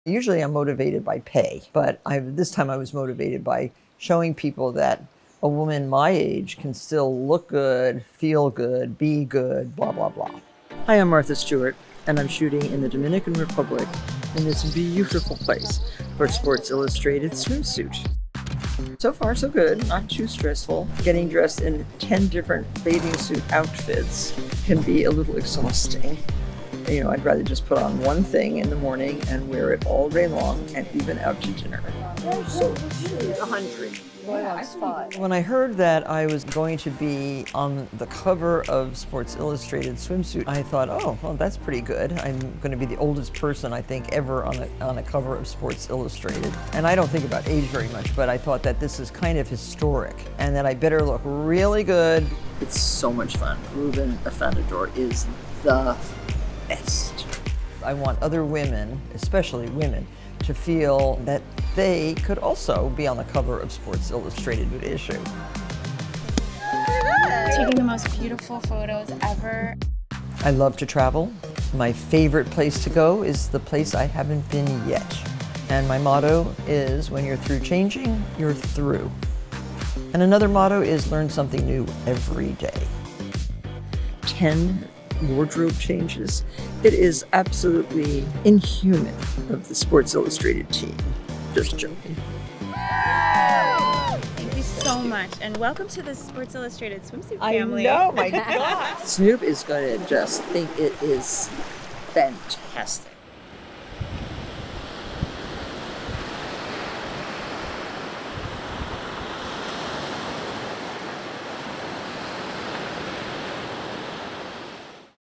Martha Stewart’s 2023 SI Swimsuit Cover Photo Shoot
Martha Stewart doesn’t ever think about age, but it did cross her mind when she learned that, at 81, she’d be the oldest model to ever land the cover of the SI Swimsuit Issue. The mother, grandmother, Emmy Award–winning television show host and founder of Martha Stewart Living Omnimedia, who has published nearly 100 books throughout her illustrious career, graces the cover of the 2023 SI Swimsuit Issue.

Hair: DJ Quintero
Makeup: Daisy Nicole Toye
Photographer: Ruven Afanador

Follow SI Swim on social!